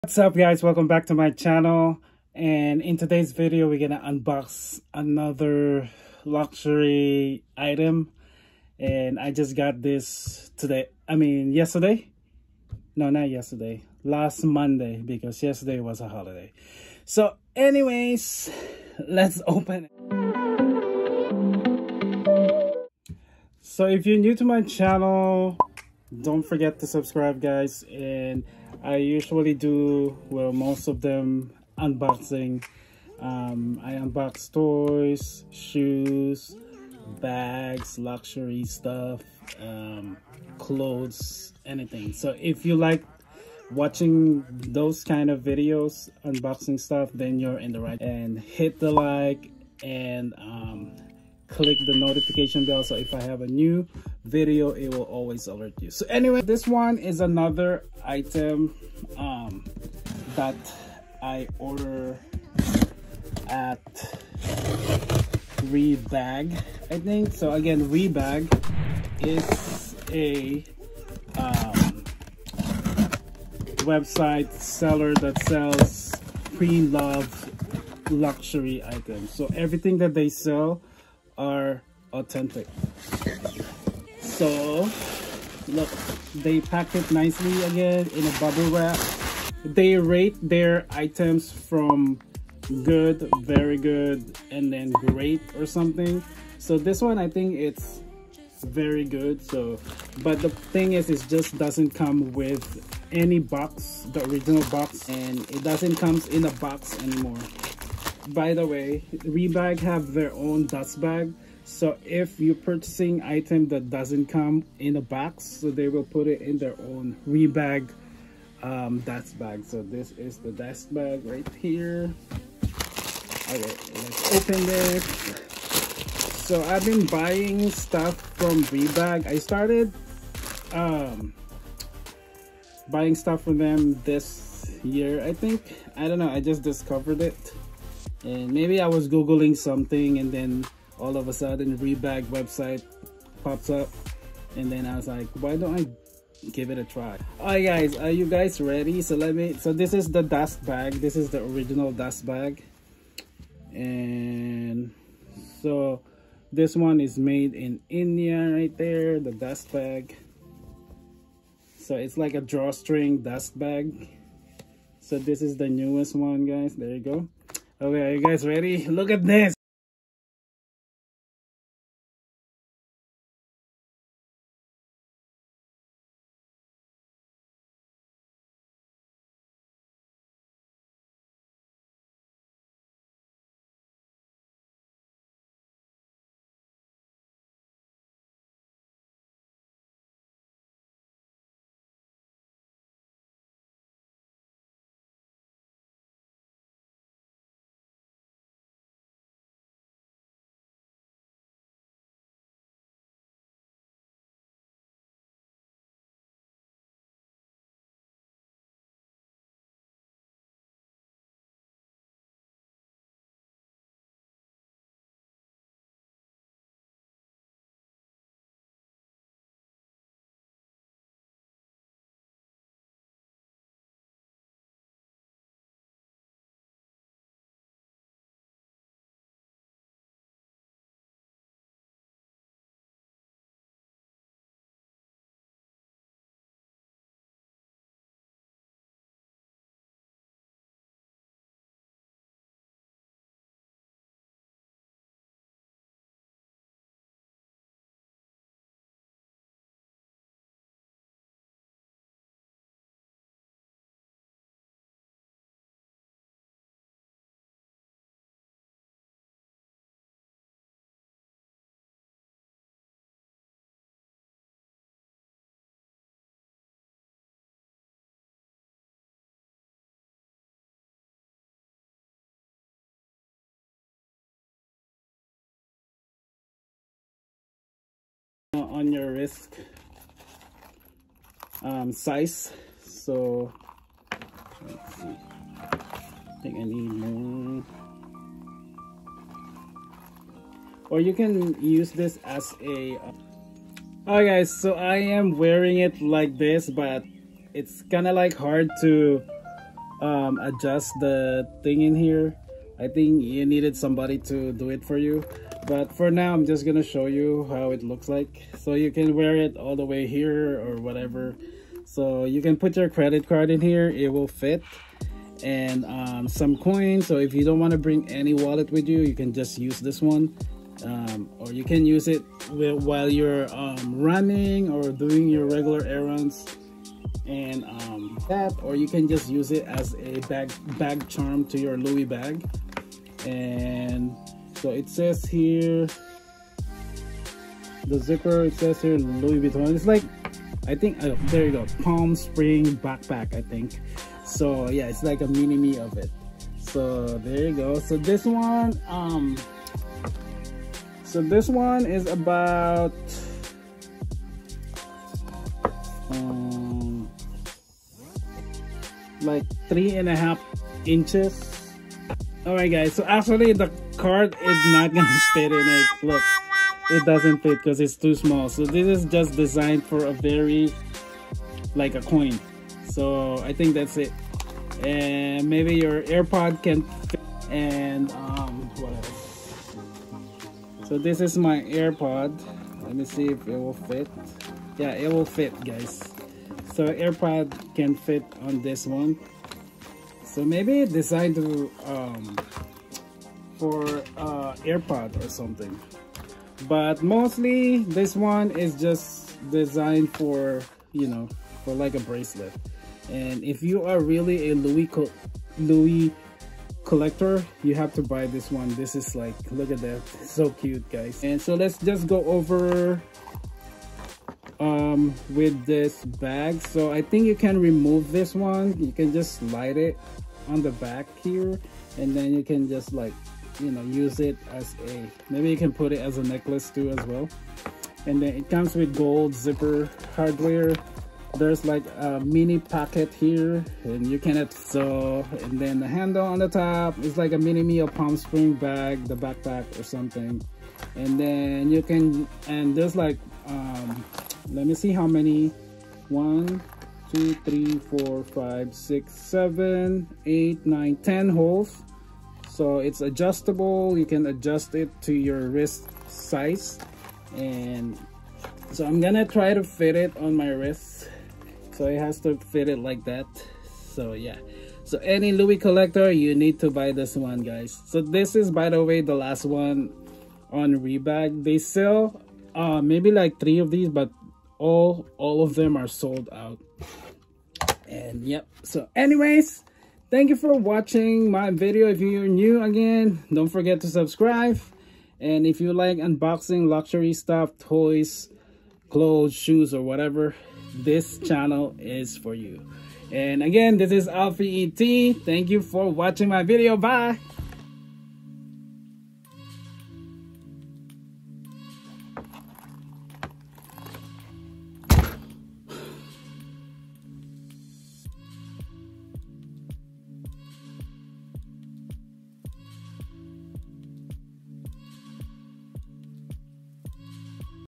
Unboxing Louis Vuitton Party Palm Springs bracelet | another Rebag.com item , but CC does not fit 😞
Hey guys another LV purchase from Rebag.com. This one is very unique. IT’s unisex, and  any body can wear this bracelet. It’s a little bit loud because of the Lv brand but ones you wear it it’s like a very special piece of jewelry even though it’s not gold or silver but it’s the brand and who you are as a collector.

Brand is Louis Vuitton visit their website at.

The name of the item is :

***Party Palm Springs bracelet

**Jeff kaale **